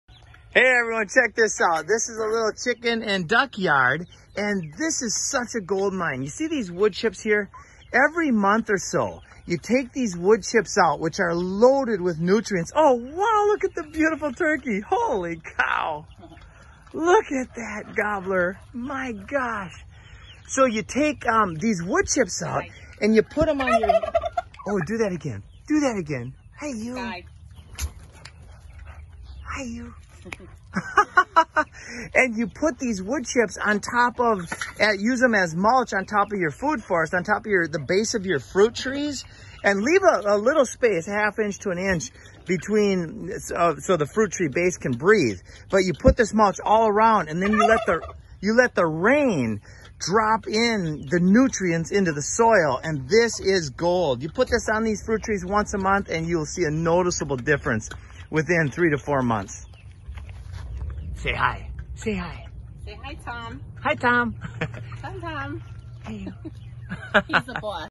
Chicken, duck, turkey yard wood chip and mulch tips. Gobble goggle.
Food Forest Eden inspiring sustainability and solutions with Food Forest Abundance.

We inspire people to grow food.